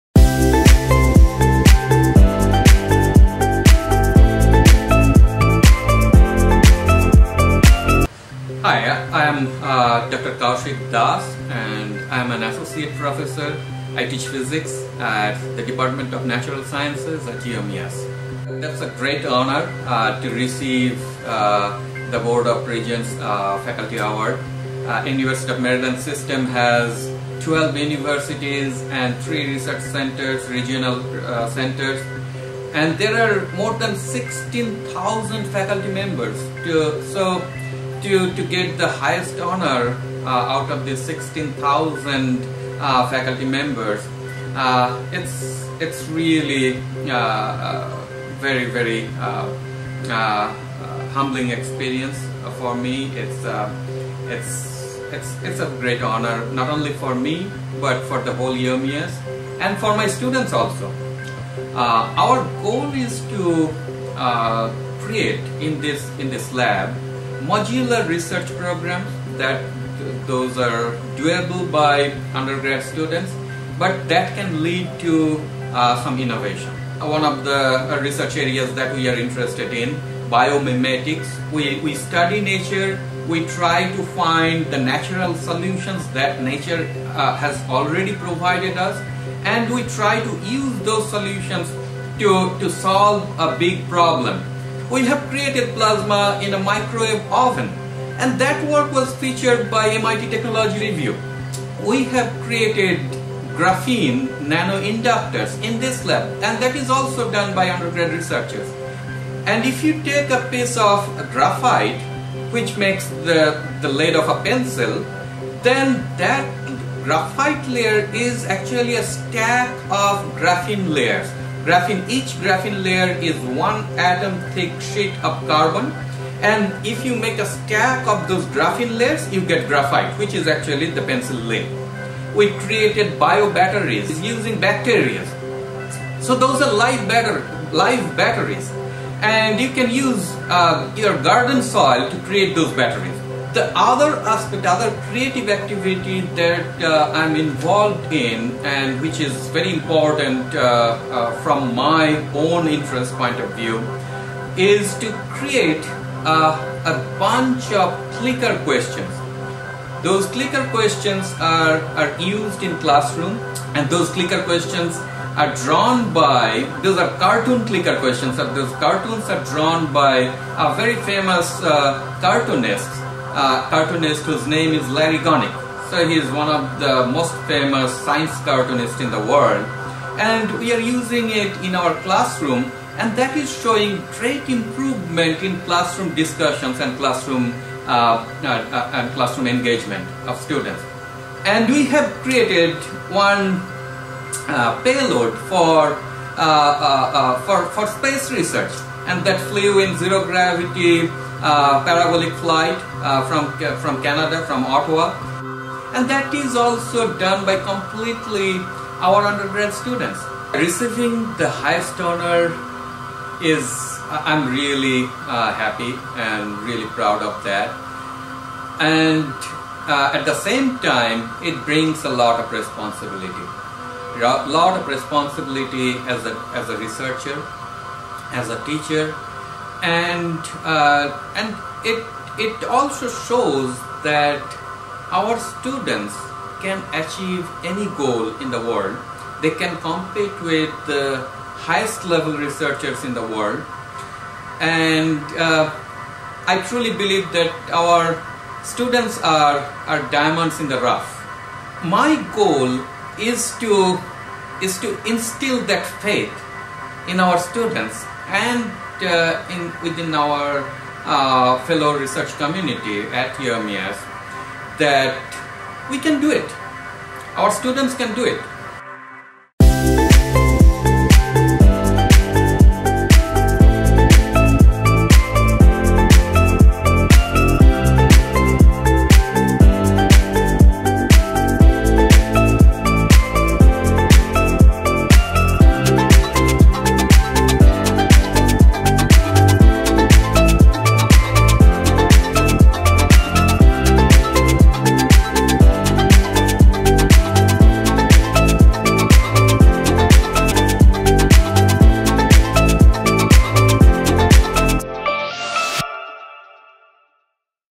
Das receives USM 2021 Regents' Faculty Award
Dr. Kausik Das, an associate professor of physics in the UMES Department of Natural Sciences, is among an elite group of faculty members within the University System of Maryland recognized for contributions to their individual institutions.